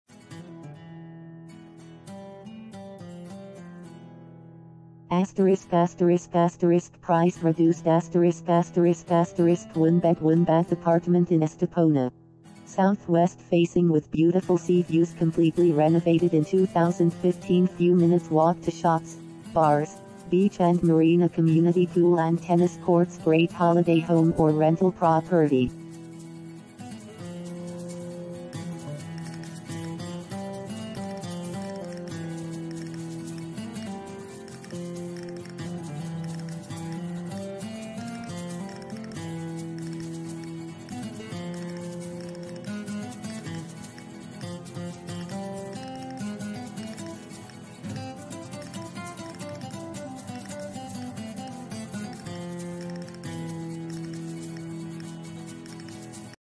1 Bedroom Middle Floor Apartment in Estepona, Costa Del Sol FOR SALE &euro; 165,000
1 Bedroom Middle Floor Apartment in Estepona, FOR SALE &euro; 165,000
***PRICE REDUCED***
1 Bed 1 Bath Apartment in Estepona

• South West facing with beautiful sea views
• Completely renovated in 2015
• Few minutes walk to shops, bars, beach and marina
• Community pool & tennis courts
• Great holiday home or rental property!
Covered Terrace
Town;Close To Shops;Close To Sea